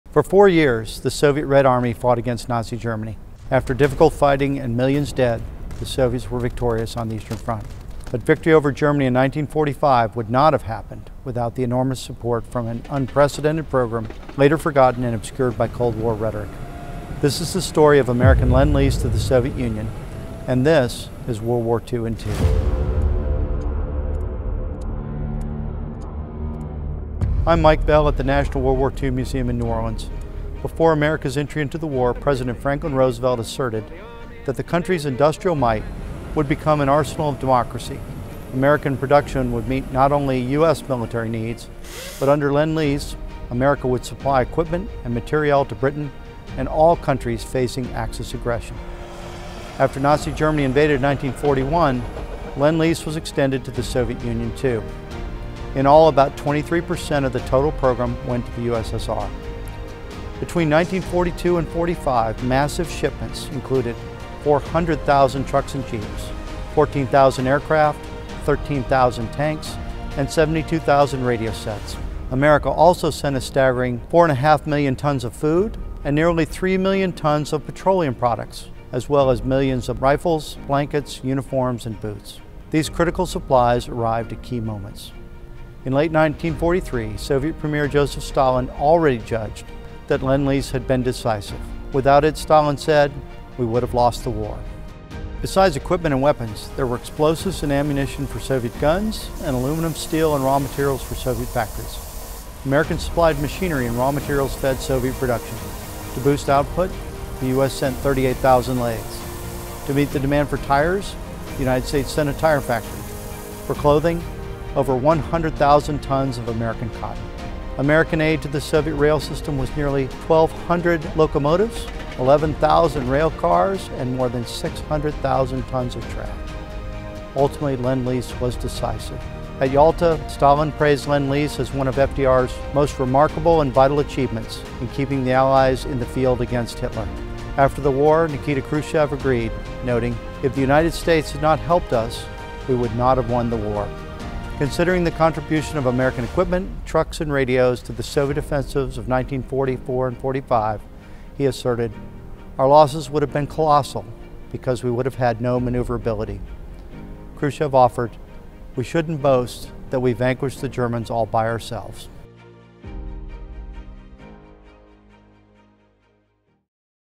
Lend Lease to the Eastern Front | WWII In 2
Despite being overlooked in many circles, American “Lend-Lease” support sent to the USSR not only tipped the scales in Eastern Europe but enabled the victory on the Russian Front. Michael S. Bell, PhD (COL, USA, Ret.), Executive Director of the Jenny Craig Institute for the Study of War and Democracy, explores the forgotten story of American aid on the Eastern Front.